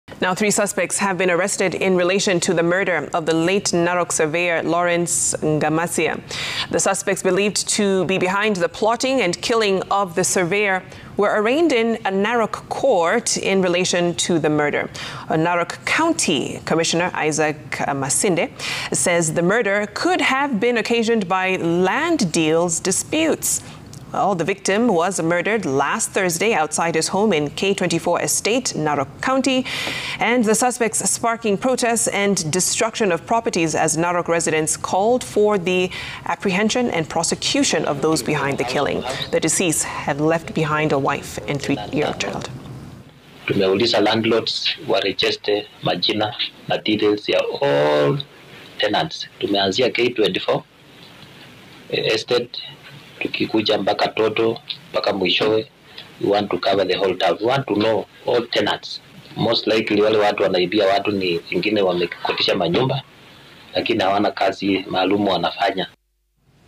Police arrest 3 suspects in the murder of land surveyor in Narok
Three suspects have been arrested in relation to the murder of the late Narok Surveyor Lawrence Ngamasia. The suspects believed to behind the plotting and killing of the surveyor were arraigned in a Narok court in relation to the murder.